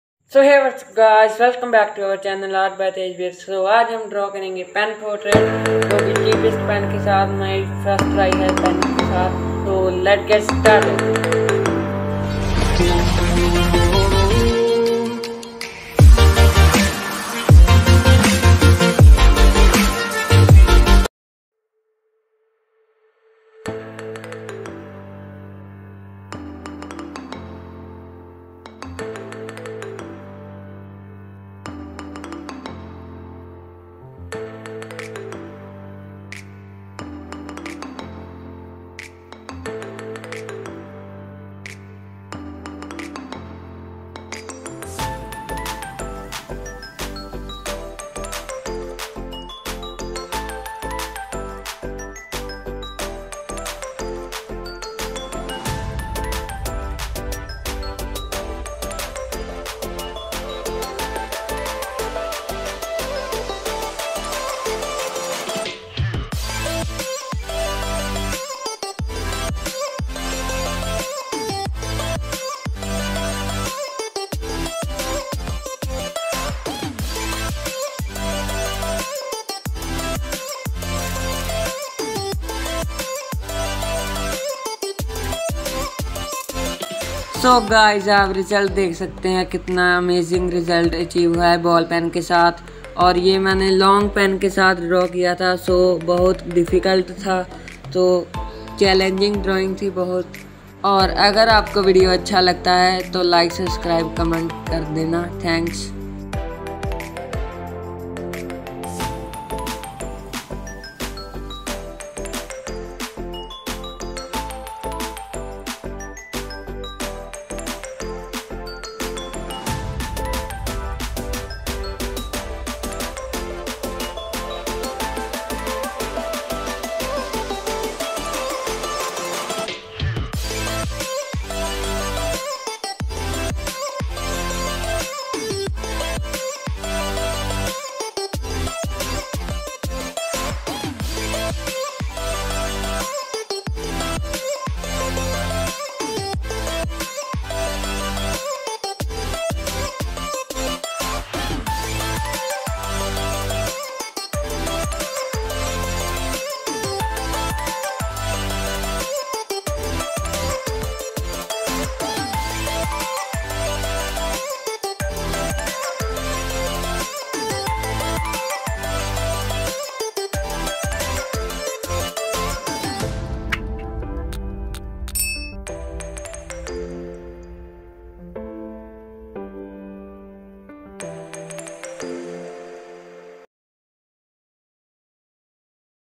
Can I draw @Mythpat portrait for congratulations for 10 million subscribers with long cheapest pen.
In this video I have drawn realistic Mythpat portrait with long pen since I haven't use pen for portrait this is my first try with  pen . Cheapest I had use .

Thanks ❤️